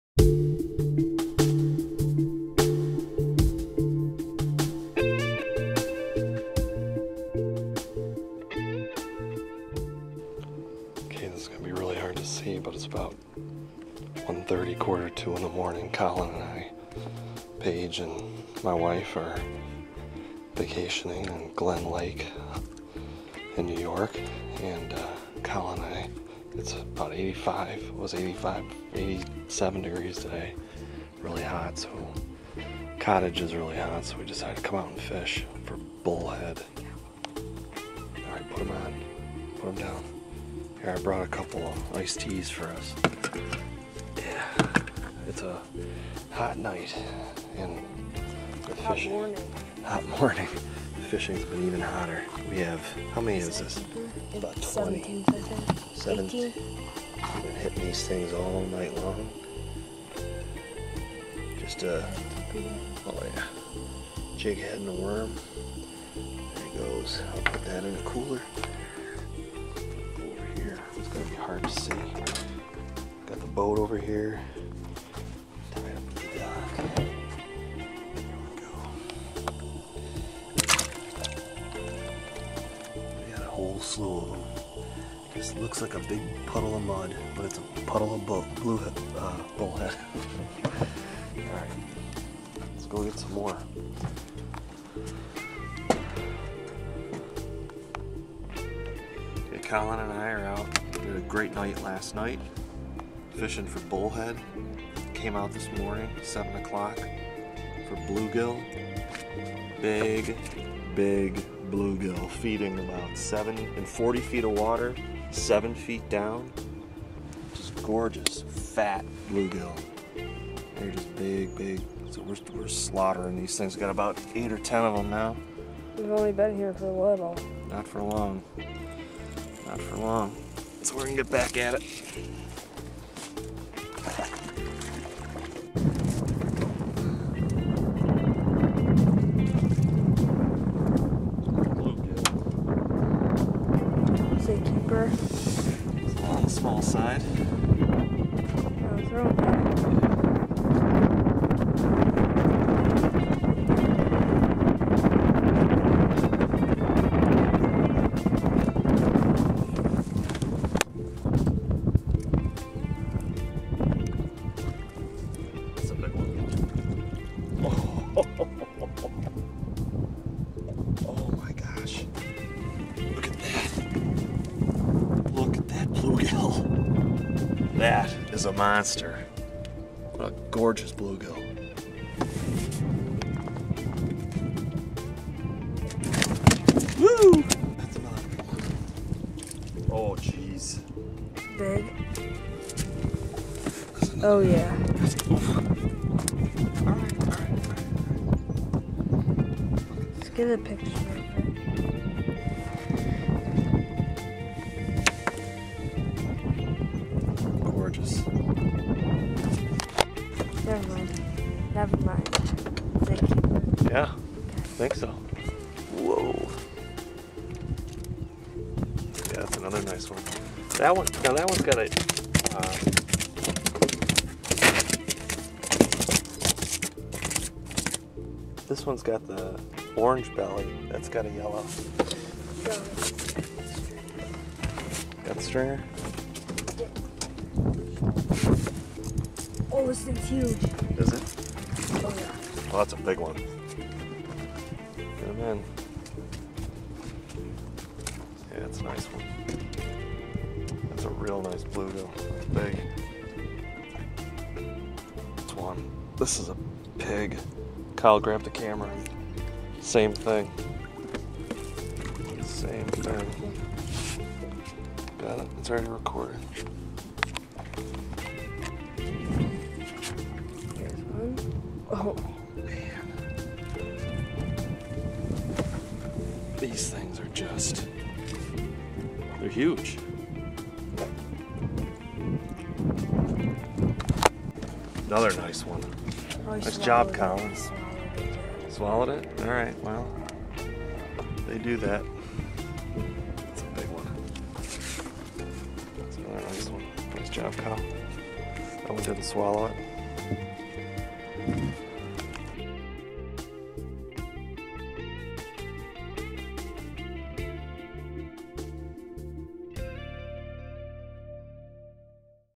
Glen Lake Adirondack Mountains Bullheads & Bluegill
Vacation at Glen Lake in the Adirondack Mountains.  This was one of the best vacations I've ever taken the family on.  Relaxing and tons of fish.